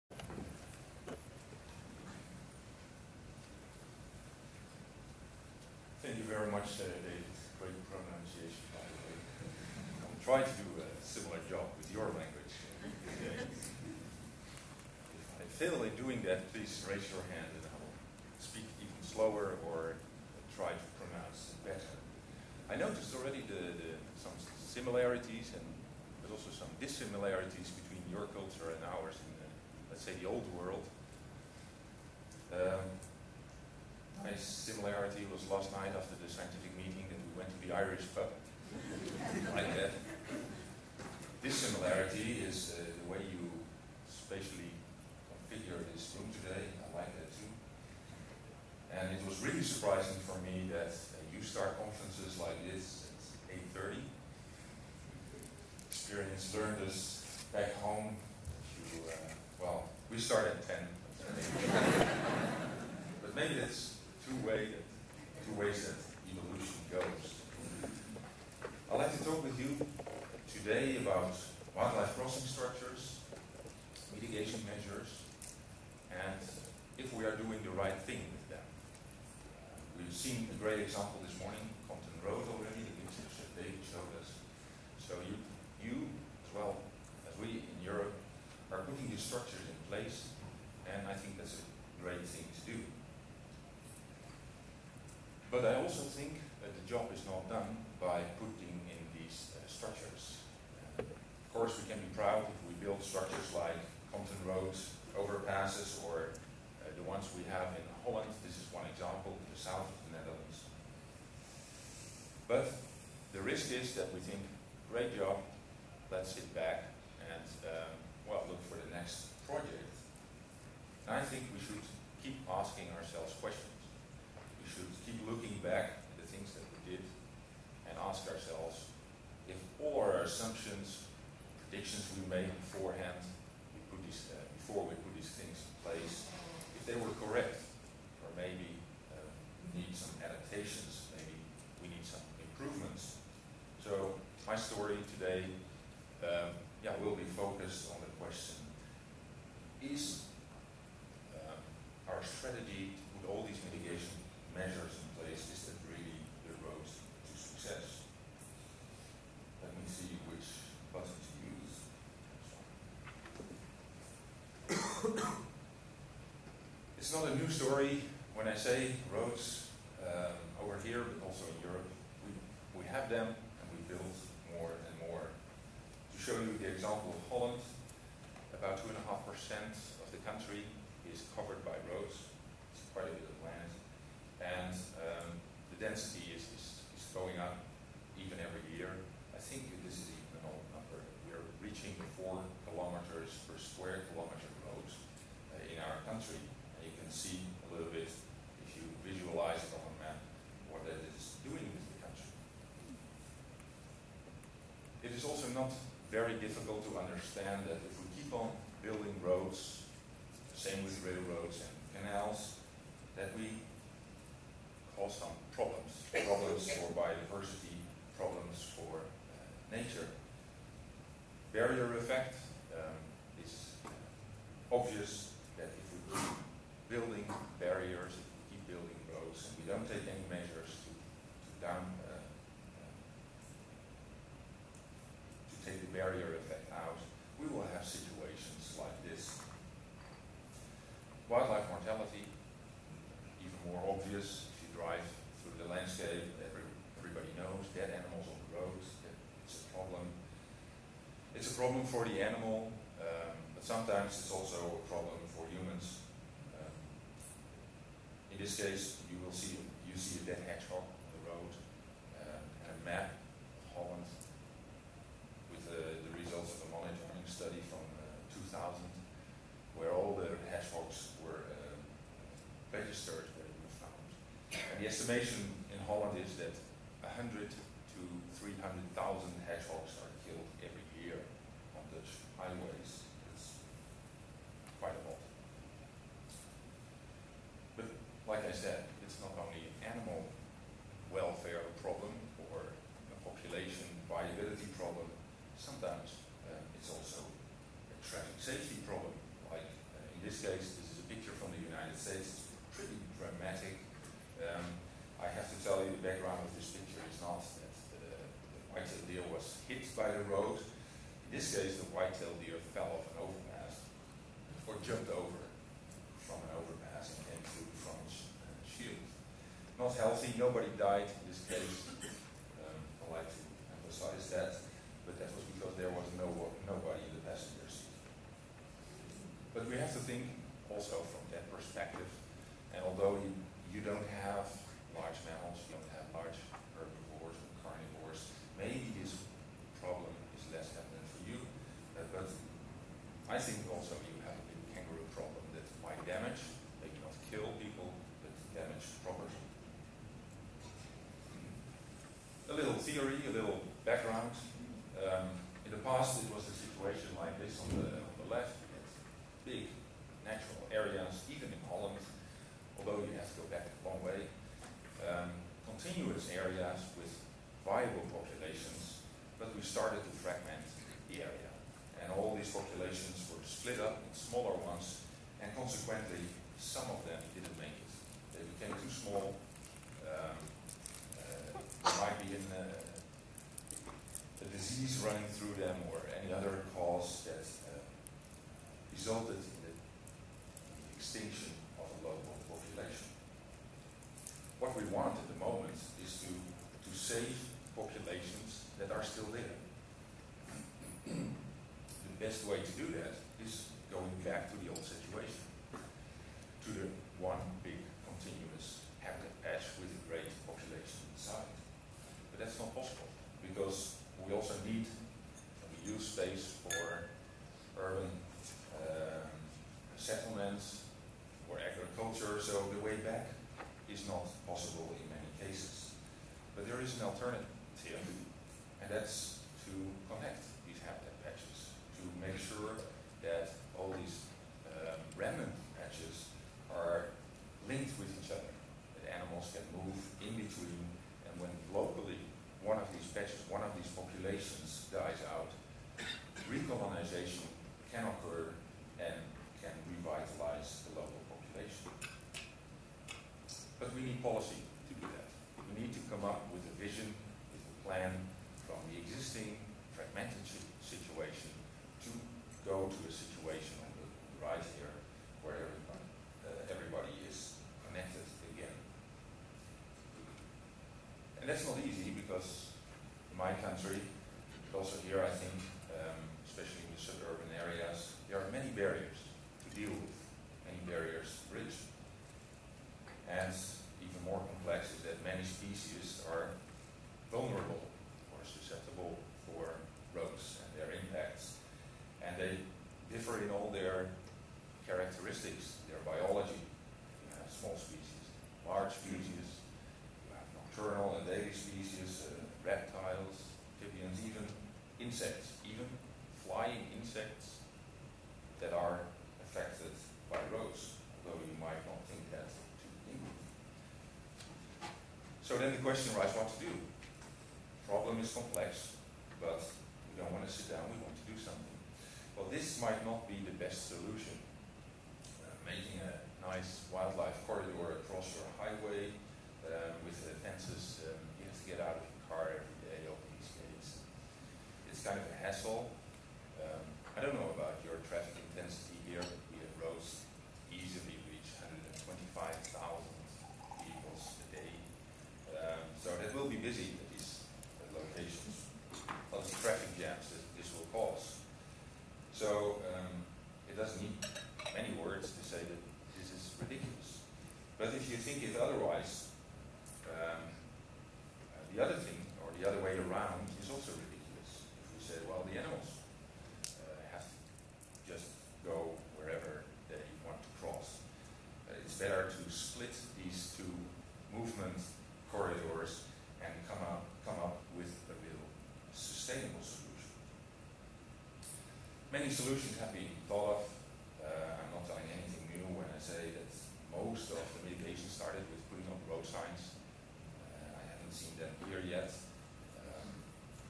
Wildlife Crossing Structures: The Road to Success Dr Edgar van der Grift, Part 1
Environment Institute of Australia and New Zealand's National Symposium about finding engineering solutions to the ecological problems of linear infrastructures such as road and rail.

Roads and railroads may have a severe impact on wildlife. They have the potential to cause mortality in wildlife, disrupt animal movement and affect both the amount and quality of wildlife habitat. Consequently, transport networks can potentially jeopardize the long-term persistence of wildlife populations, communities and ecosystems. Around the world, road administrations and nature conservation organisations currently spend considerable amounts of money annually on engineering solutions  such as wildlife crossing structures  to increase the permeability of transport corridors for wildlife. In the Netherlands the construction of wildlife passages increasingly becomes standard procedure when building new roads. Furthermore, in 2004 a 410 million euro defragmentation program has been launched to improve habitat connectivity across existing (rail)roads. The question that consequently rises in response to such investments is: does it work?